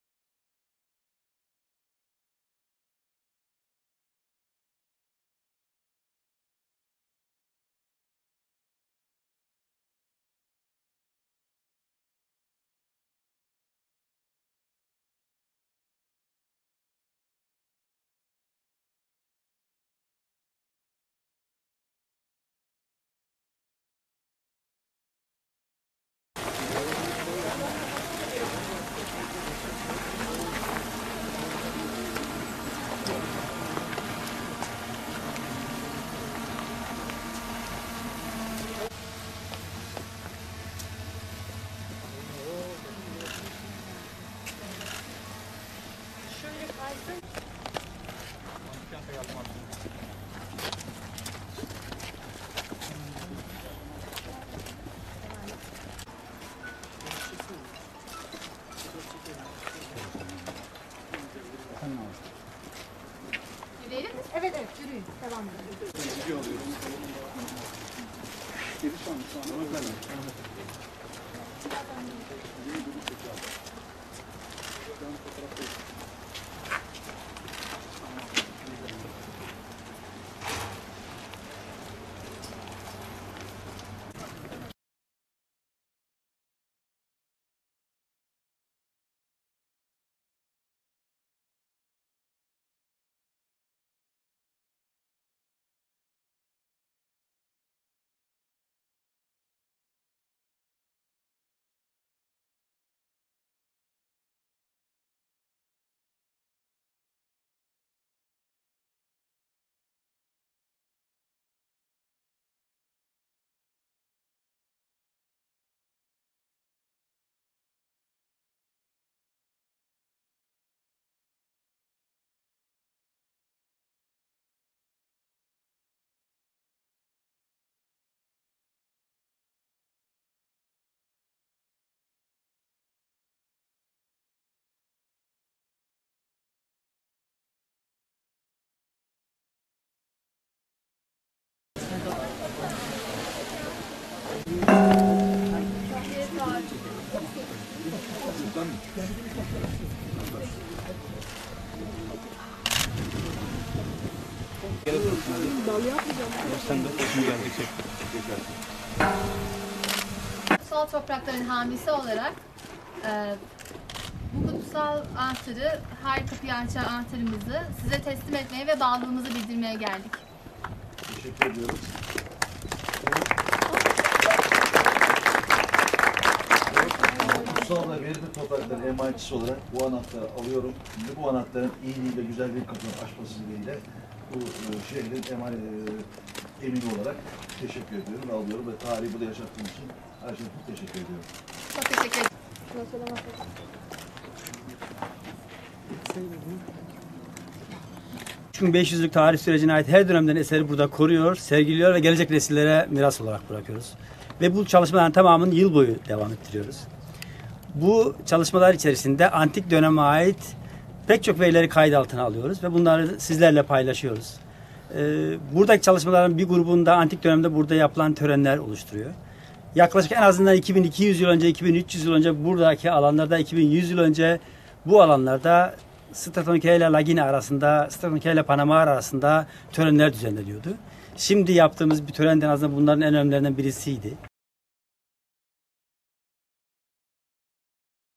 MUĞLA Stratonikeia'da öğrencilerden temsili anahtar taşıma töreni
MUĞLA'nın Yatağan ilçesinde 3 bin 500 yıl önce kurulan dünyanın en büyük mermer antik kenti Stratonikeia'da, üniversite öğrencileri tarafından temsili anahtar taşıma töreni gerçekleştirildi.\r\n\r\nAntik dönemde, dini merkez Lagina'dan siyasi merkez Stratonikeia'ya bağlılıklarını bildirmek amacıyla gerçekleştirilen Hekatesia anahtar taşıma töreni geleneği Muğla Sıtkı Koçman Üniversitesi Kültürel Miras Topluluğu öğrencileri tarafından sembolik olarak yeniden canlandırıldı.
 Muğla Büyük Şehir Belediyesi, İl Kültür ve Turizm Müdürlüğü, Sıtkı Koçman Üniversitesi, Yatağan Kaymakamlığı, Yatağan Belediye Başkanlığı ve Stratonikeia-Lagina Kazı Başkanlığı katkılarıyla düzenlenen yürüyüş, Stratonikeia Antik Kenti'nin kuzey kapısından başladı. Öğrenciler, Bouleuterion (Meclis Binası)'nda protokol üyeleri tarafından karşılandı. Yatağan Belediye Başkanı Mesut Günay'a anahtarı teslim eden öğrenci, Lagina dini merkezin Stratonikeia siyasi merkeze bağlılığını bildirdi.\r\n\r\nStratonikeia Antik Kenti Kazı Heyeti Başkanı Prof. Dr. Bilal Söğüt, \"3 bin 500 yıllık tarihi döneme ait her eser burada korunuyor, sergiliyor ve gelecek nesillere miras bırakıyoruz. Kazı çalışmalarımızı yıl boyunca yapıyoruz. Antik döneme ait çalışmalarımızı kayıt altına alıp, sizlerle paylaşıyoruz. 2 bin 300 yıl önce törenler düzenleniyordu. Yaptığımız tören ise bunların en önemlilerinden birisiydi\" dedi.\r\n\r\nHaber-Kamera: Cavit AKGÜN / MUĞLA, (DHA)\r\n